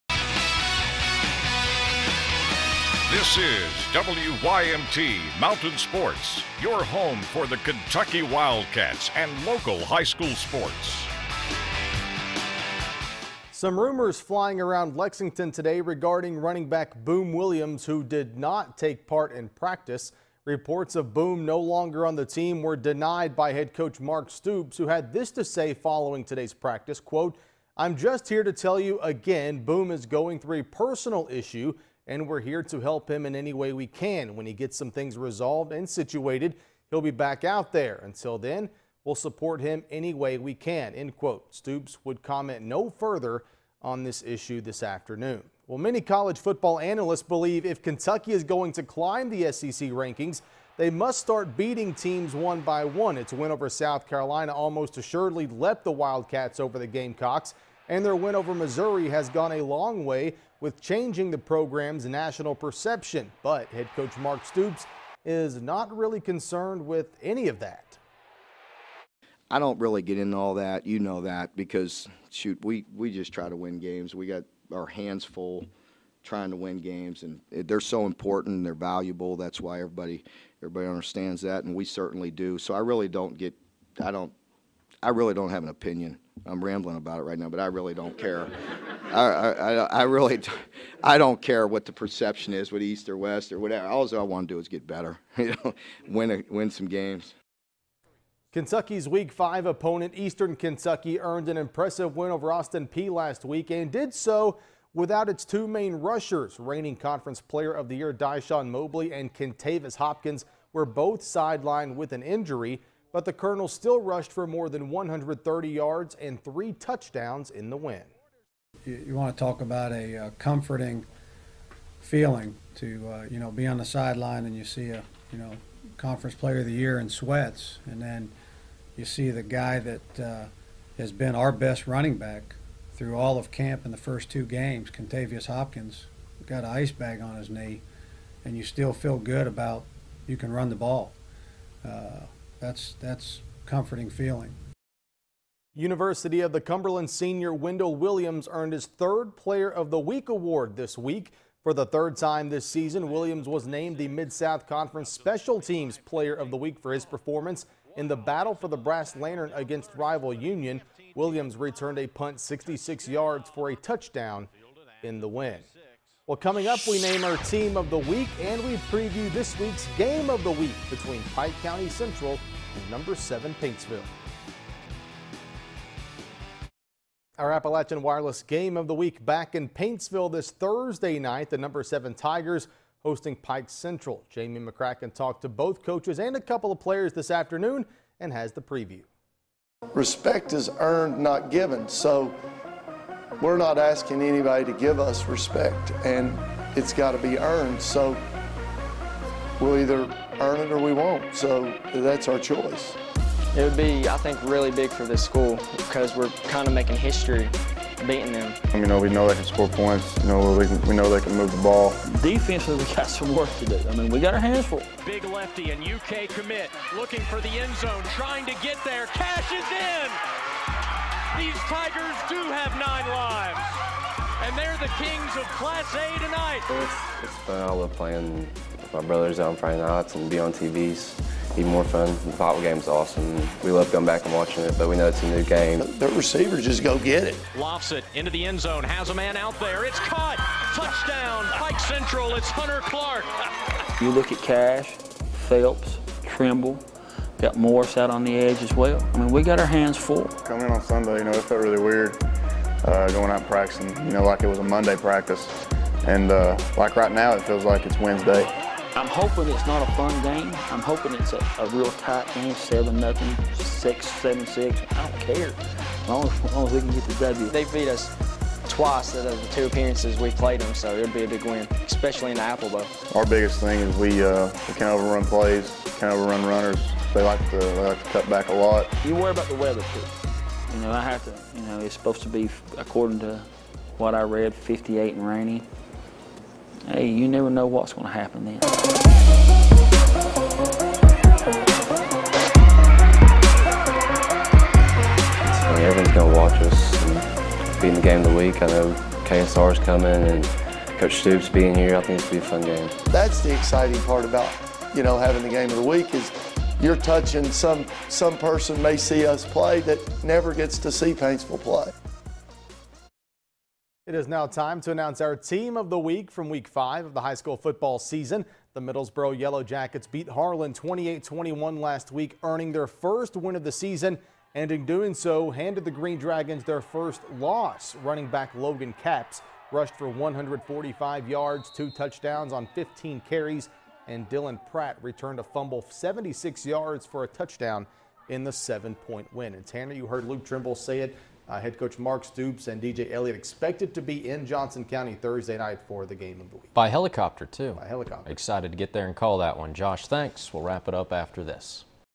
9/29/15 - 11 p.m. - WYMT Sports
Kentucky Football
Boom Williams, EKU Football
Team of the Week - Middlesboro
Game of the Week Preview - Pike Central at Paintsville